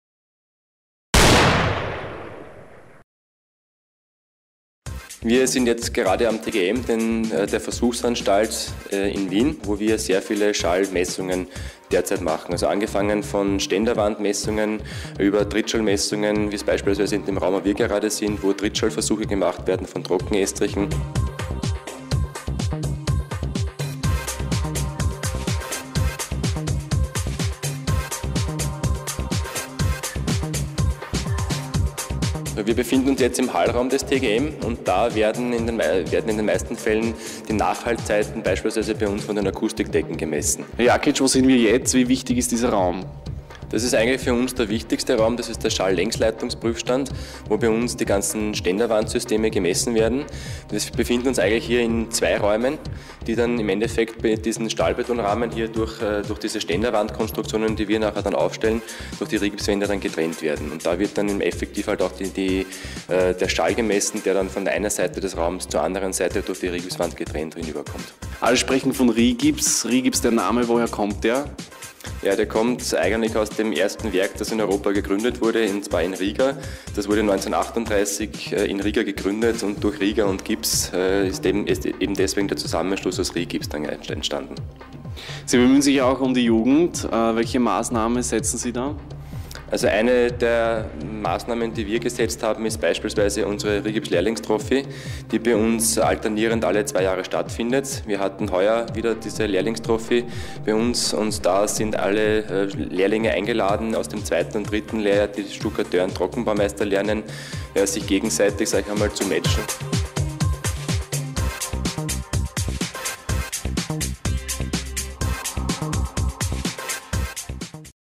StunT Team in der Versuchsanstalt des TGM
Das StunT-Team war wieder für euch unterwegs.
Diesmal in den Versuchsanstalten des TGM Wiens im 20. Wiener Bezirk. Wir haben uns die Schallmessungen in den Hallräumen genauer angeschaut. Um euch dies besser vorstellen zu können, haben wir Herrn Ing. Thomas Jakits (Leitung Anwendungstechnik Rigips) interviewt, der uns die Herkunft des Wortes „RIGIPS" verraten und uns die verschiedenen Hallräume erklärt hat.